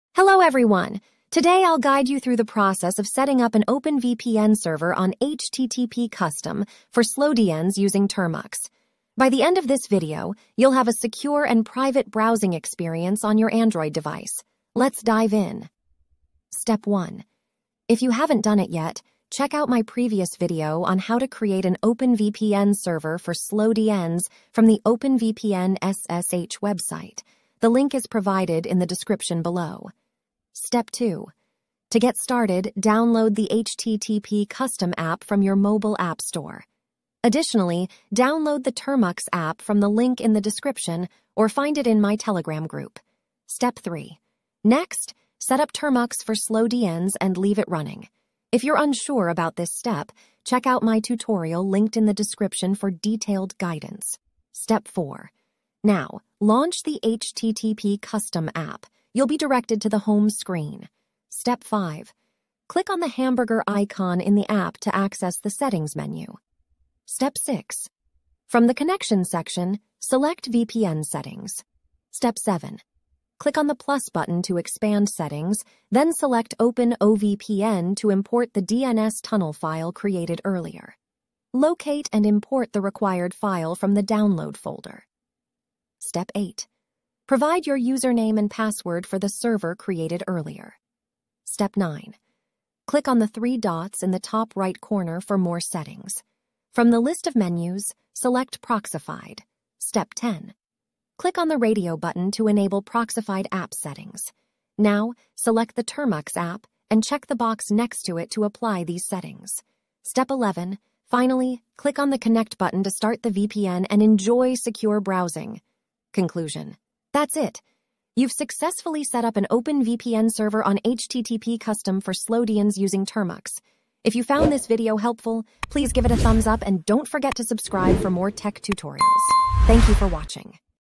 Step-by-Step: OpenVPN Server Setup on HTTP CUSTOM for SlowDns using Termux
In this video, I'll walk you through the step-by-step process of setting up an OpenVPN Server on HTTP CUSTOM for SlowDns using Termux. With this setup, you'll have a secure and private browsing experience on your Android device. Follow the instructions carefully to ensure a smooth setup.